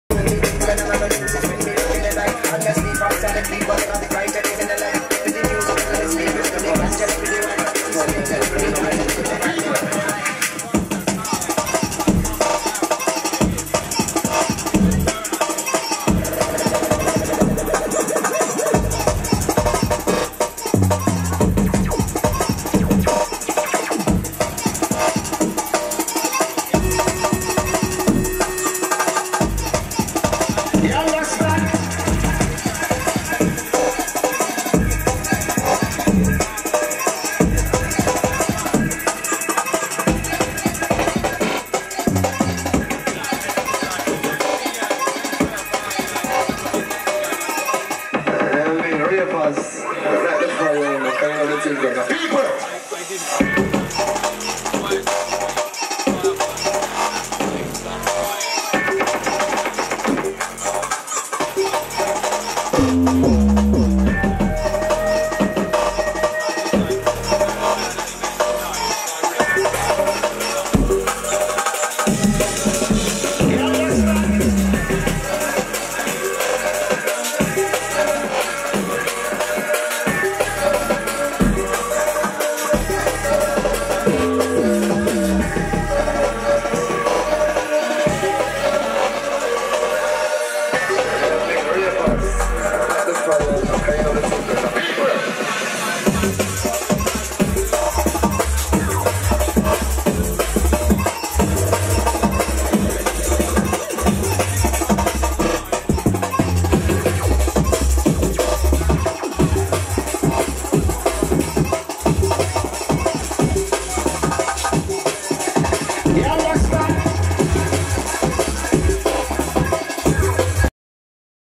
Mrs Magoo (Live from The Messy)
A wee clip from Mrs Magoo doing her thang at The Messy , Summer 2021.